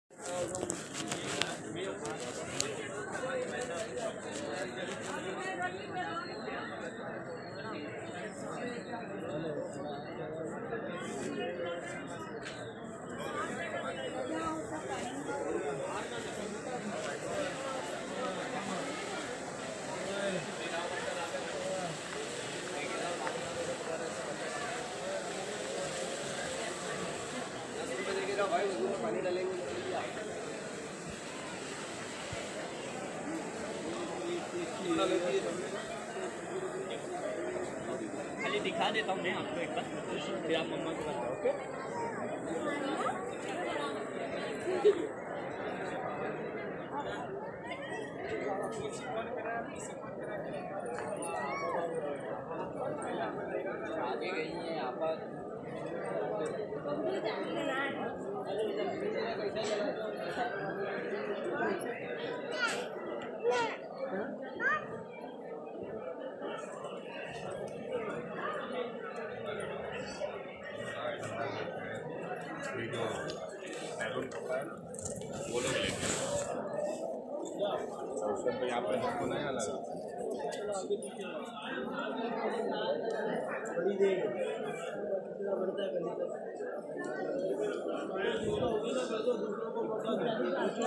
What is Badi deg? Why is Ajmer Dargah famous?
How many degrees are there in Ajmer Sharif?

What is Badi deg?

Why is Ajmer Dargah famous?

What is the name of Ajmer Sharif